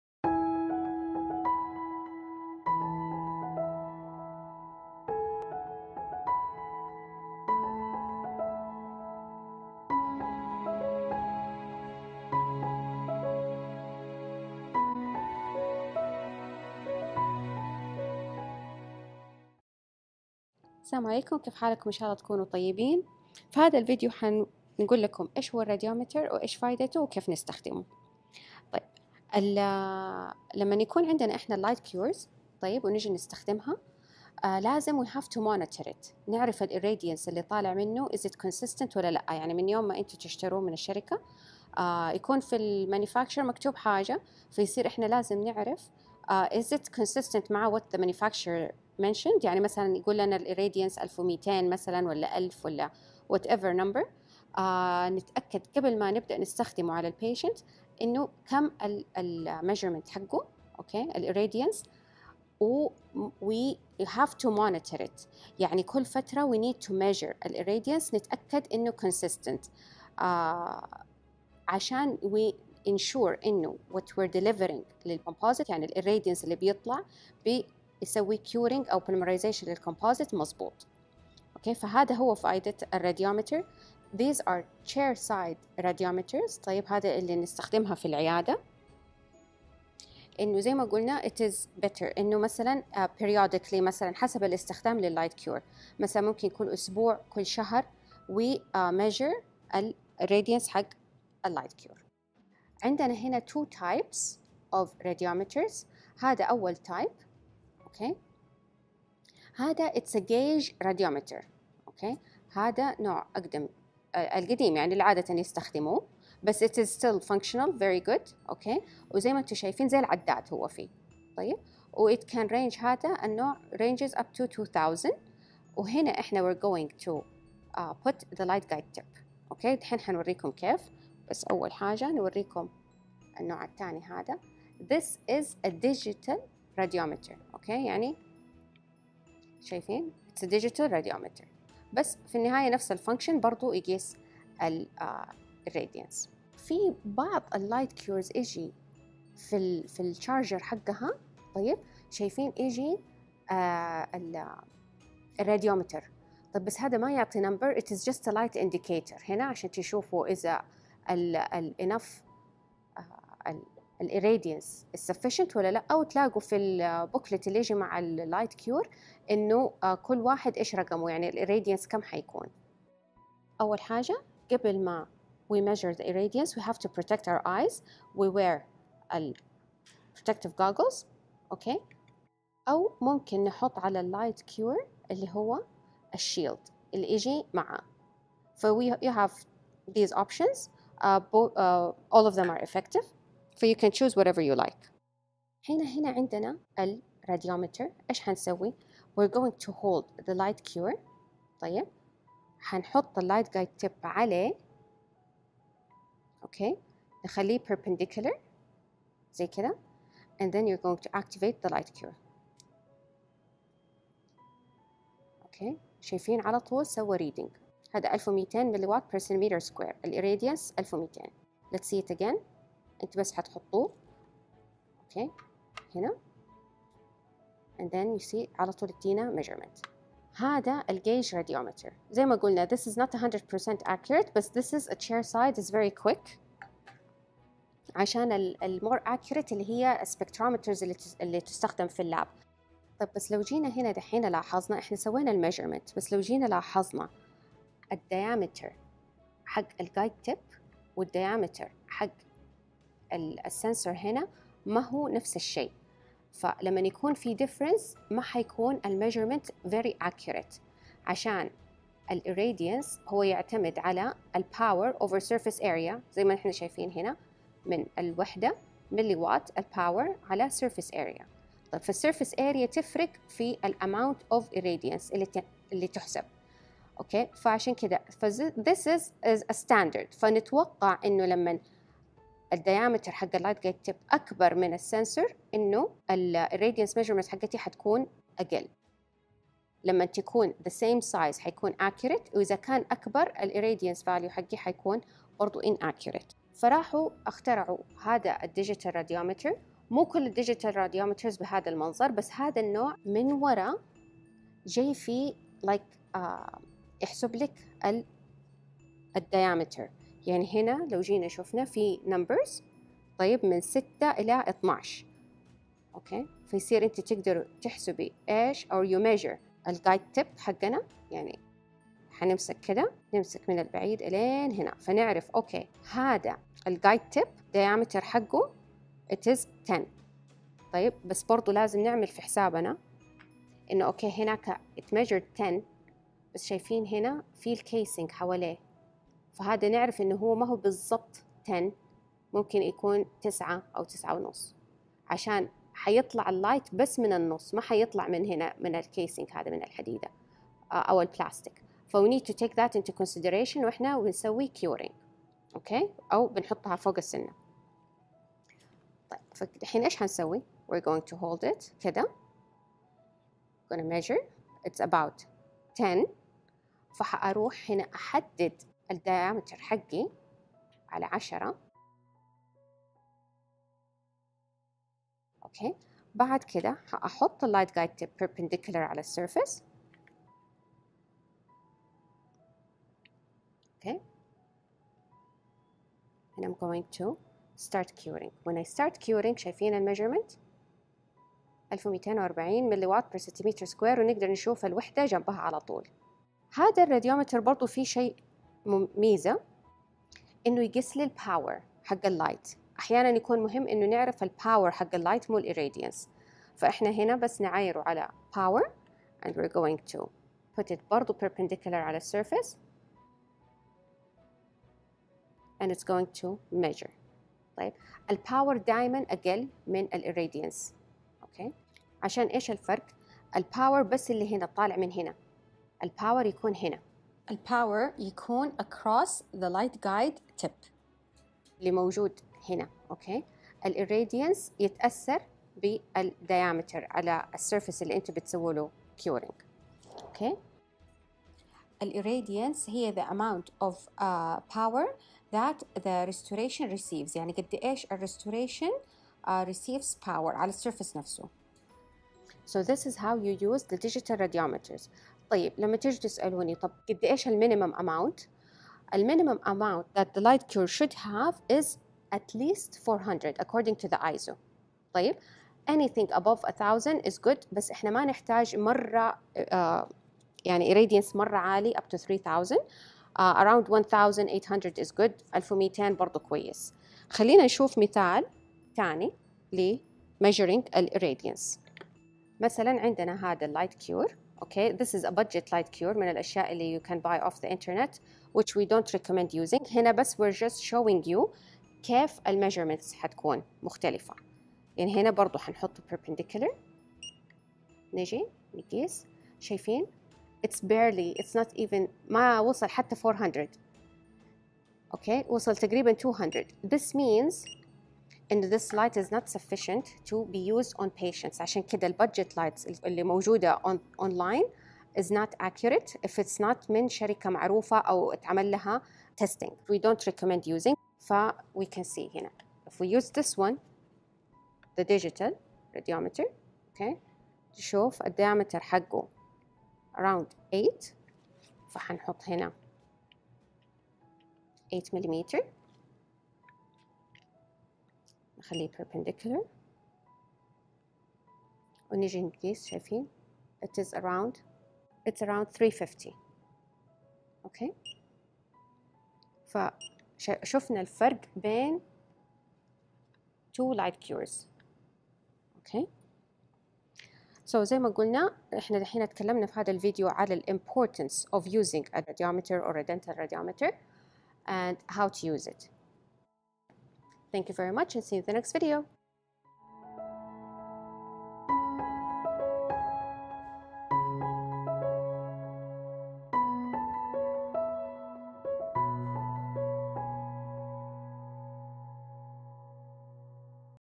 Dental light-curing unit chair side radiometers (Types and how to use them) - Arabic عربي
Dental light-curing unit chairside radiometers (Types and how to use them)

- To learn about cavity preparation terminology in Operative Dentistry (cavity walls, line angles, and point angles), watch the video:

- To learn about restorative dental instruments including examination instruments, cutting hand instruments, non-cutting hand instruments, and rotary instruments, watch the video:

00:00 – Introduction
00:14 – Title: Light-curing units (Dental radiometers)
00:20 – Introduction to dental radiometers
01:42 – Gauge radiometer
02:14 – Digital radiometer
02:26 – Built-in radiometer
02:52 – How to use?
03:14 – Gauge radiometer: How to use?
04:57 – Digital radiometer: How to use?
07:39 – Minimum irradiance value
08:02 – Budget light-curing unit irradiance measurement